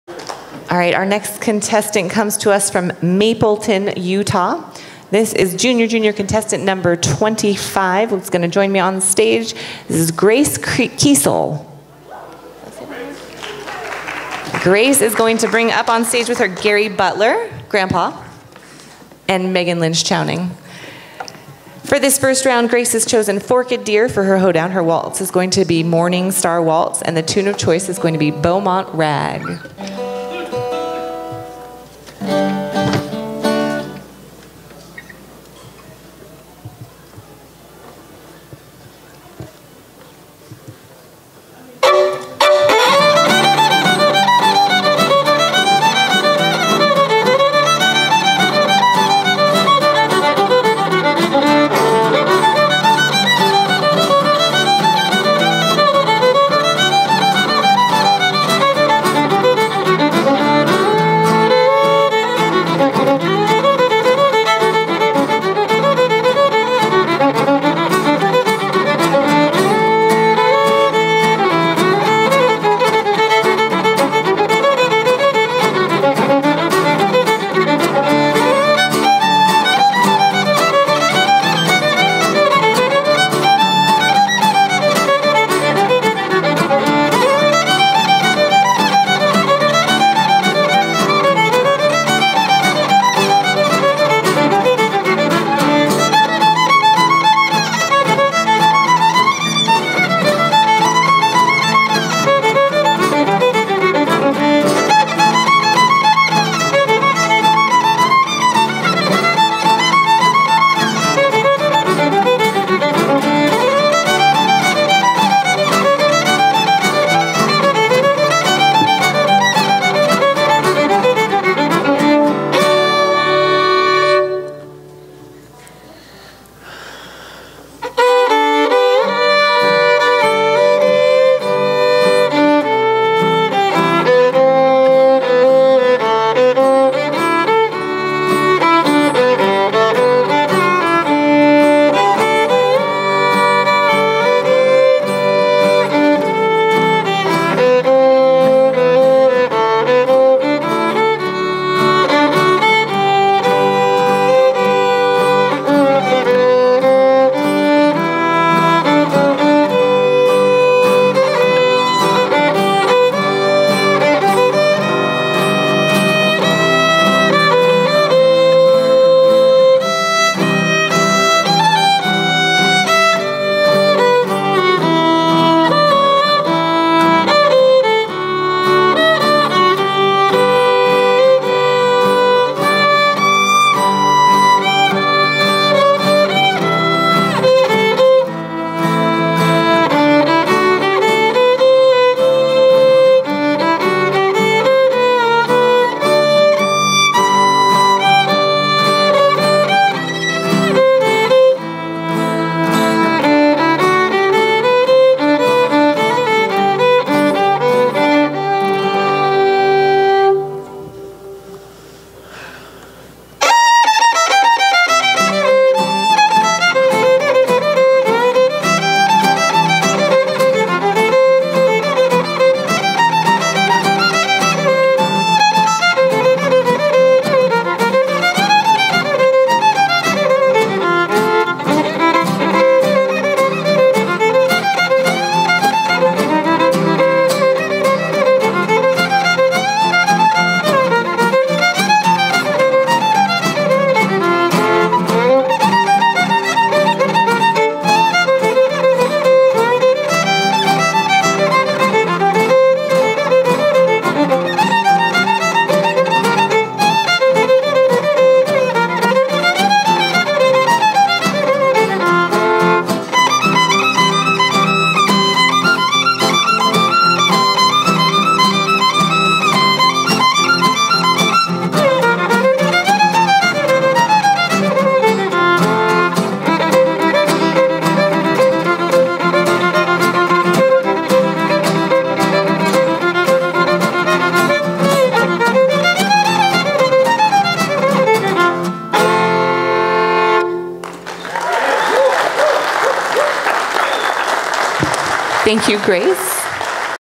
2017-06-20 JJ1 Grace Keisel - Jr Jr - Weiser Fiddle Contest 2017
Junior-Junior Fiddle Division  - Round 1
National Oldtime Fiddlers Contest & Festival  2017